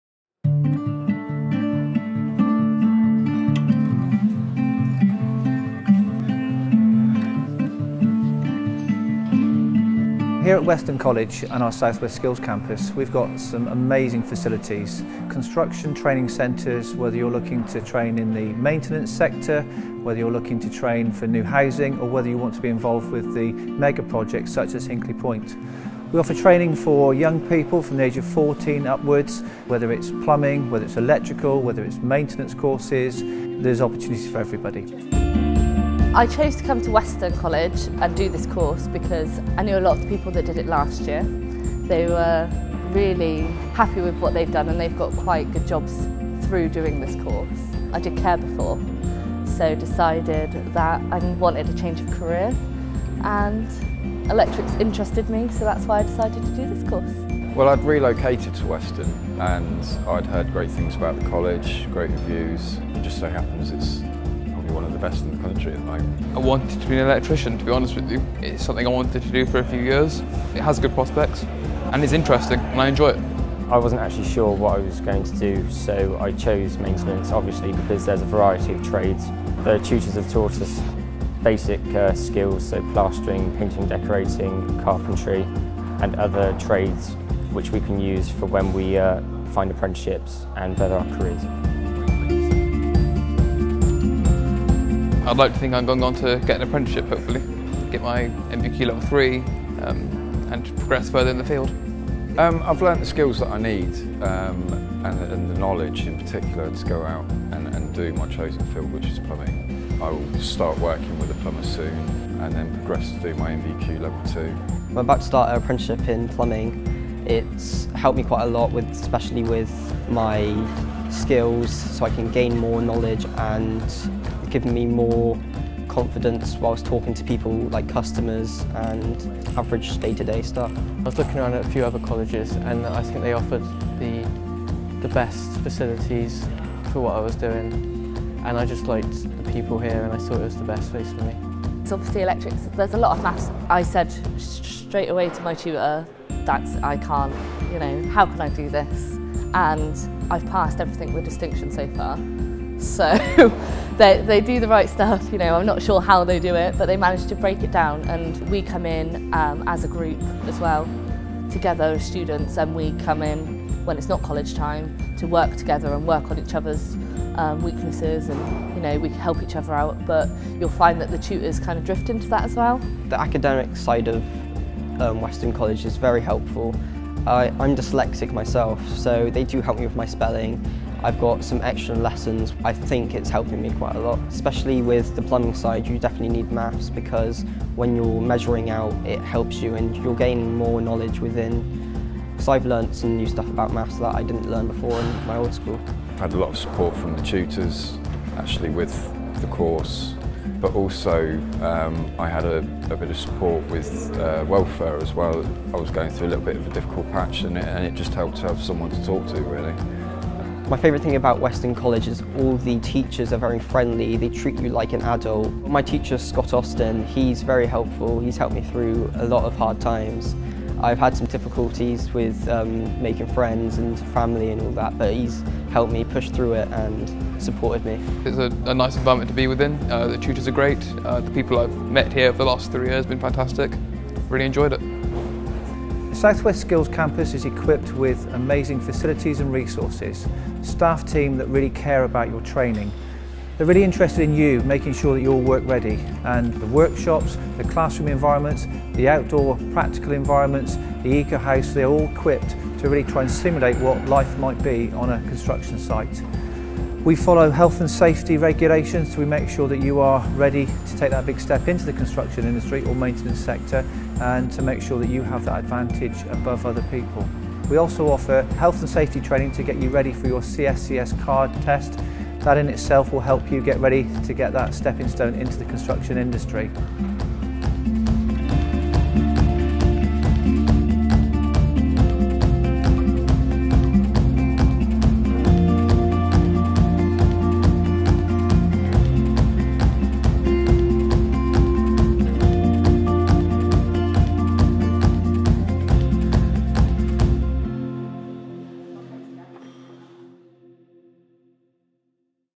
Construction & Building Services at Weston College
We deliver training in a growing range of practical construction skills including bricklaying, carpentry, electrical installation, multi-craft, property maintenance, paining and decorating, plastering and plumbing, as well as renewable energy courses. All the training takes place in our bespoke, well-equipped workshops at our South West Skills Campus.